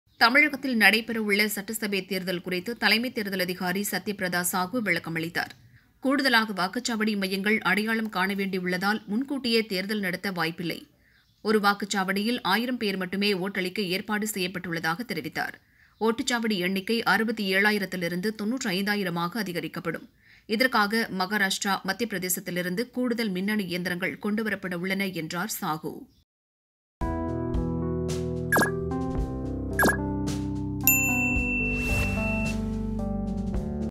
தலைமை தேர்தல் அதிகாரி சத்யபிரத சாகு விளக்கம்
தமிழகத்தில்
தேர்தல்
முன்கூட்டி
நடக்குமா?